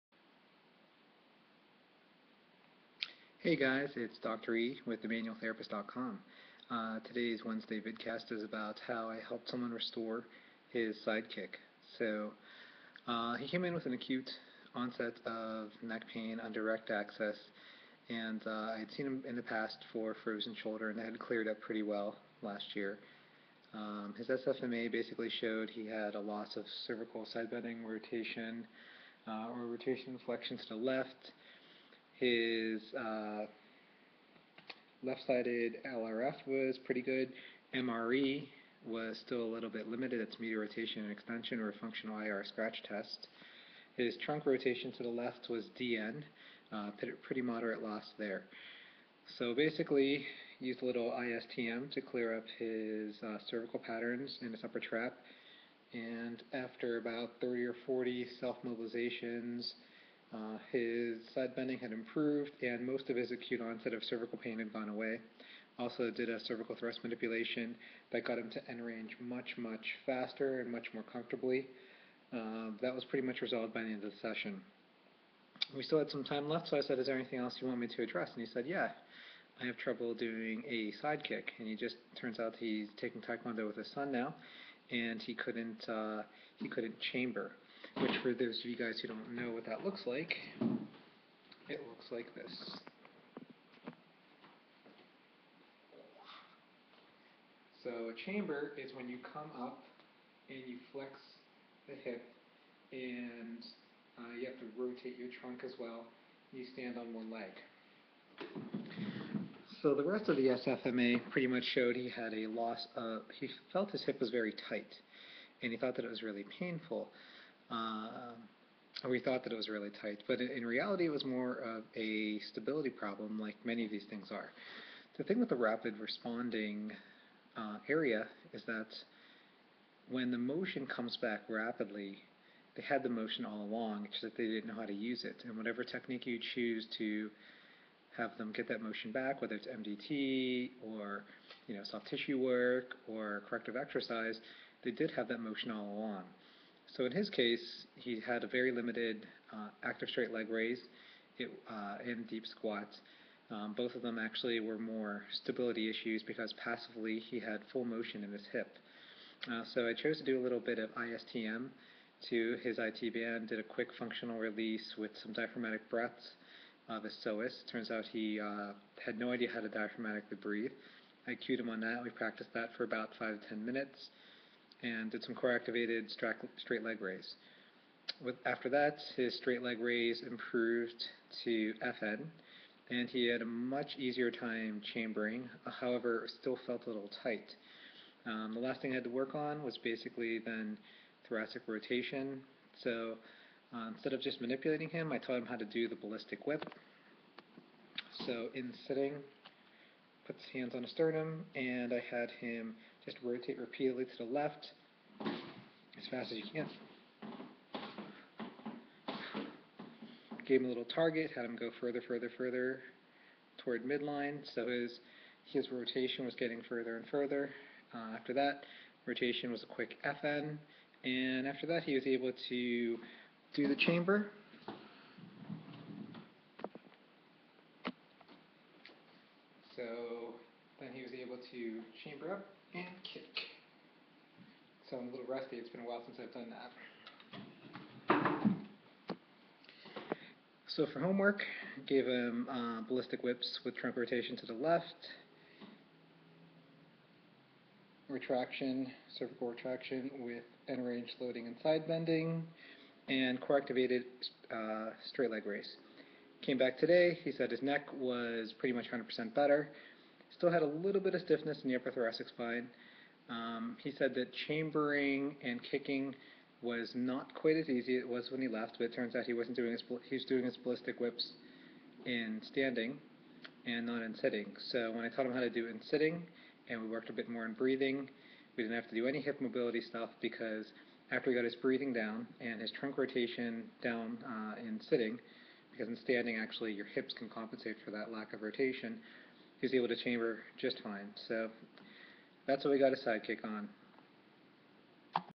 Vidcast on How I Reset a Tae Kwon Do Sidekick
Does Robin need help? No, a patient came under direct access for acute neck pain, and it turns out he also has trouble with a Tae Kwon Do, sidekick!